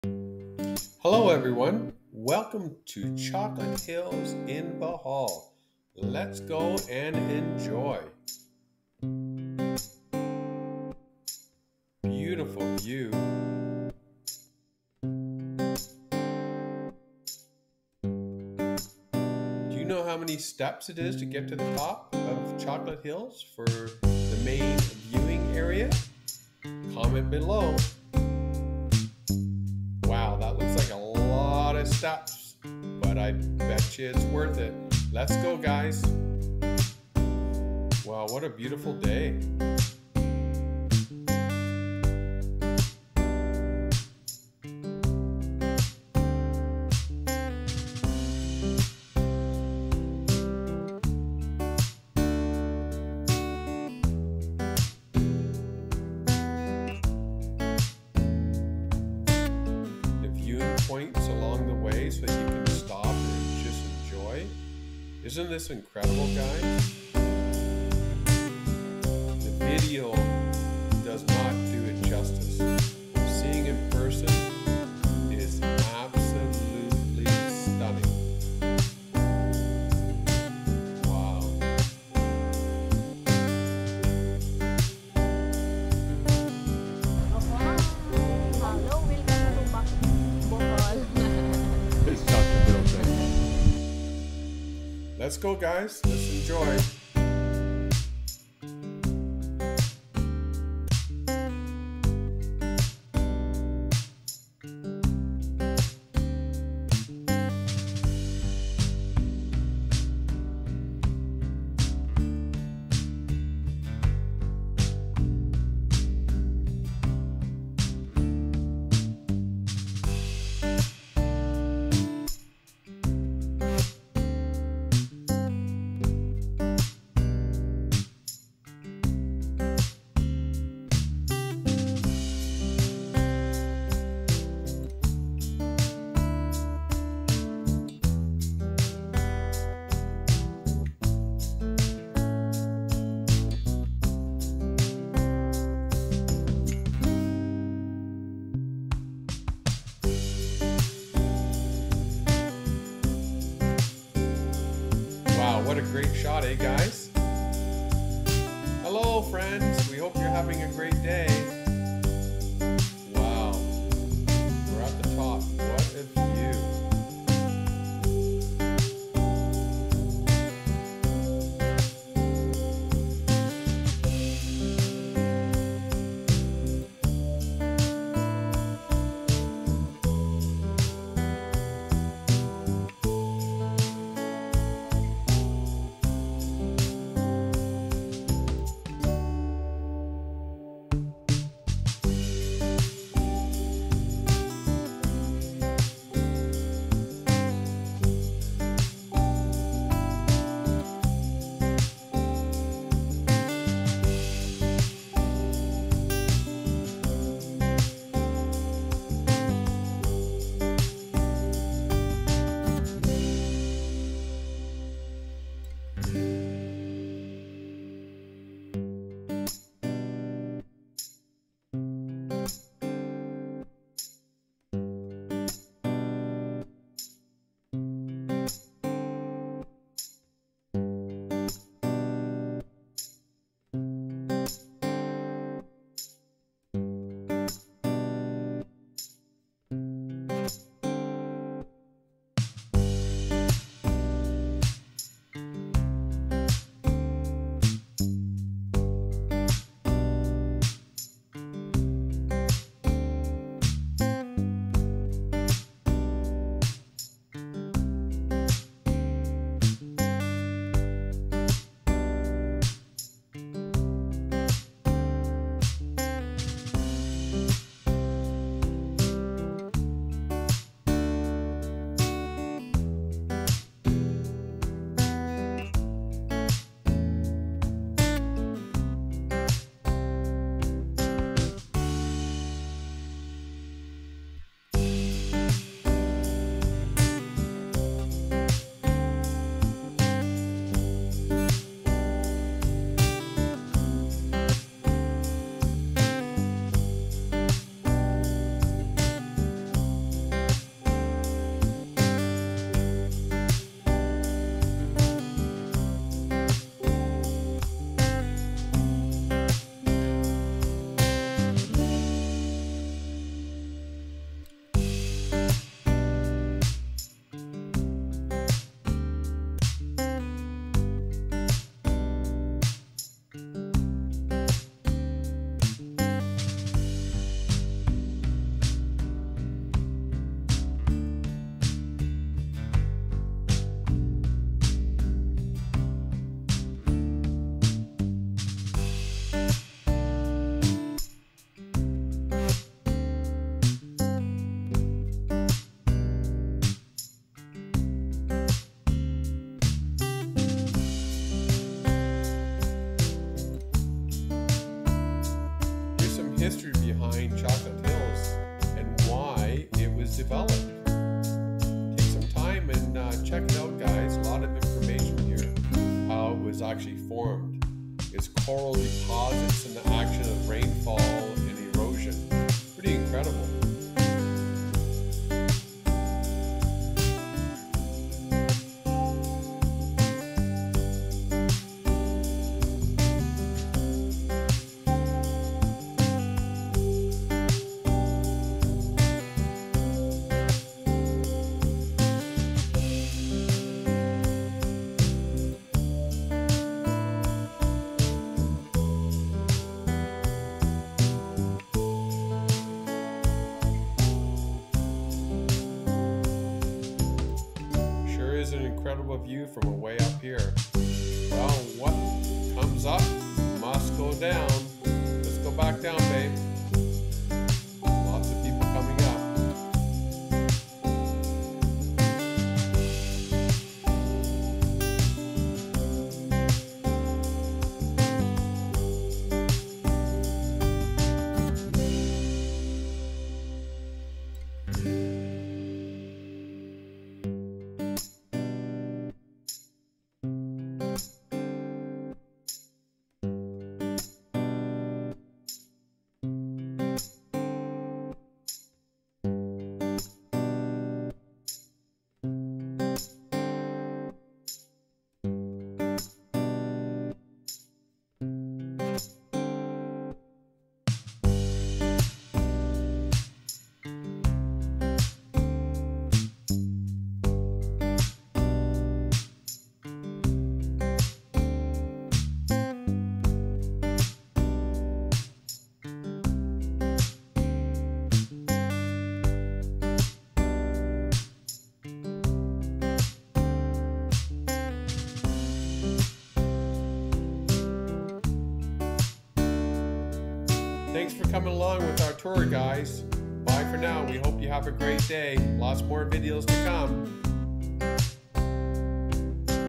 Discover The Spectacular Chocolate Hills In Bohol, Philippines - A Bucket List Destination!
CHOCOLATE HILLS on exotic Bohol Island, we walk up  214 steps to a spectacular view point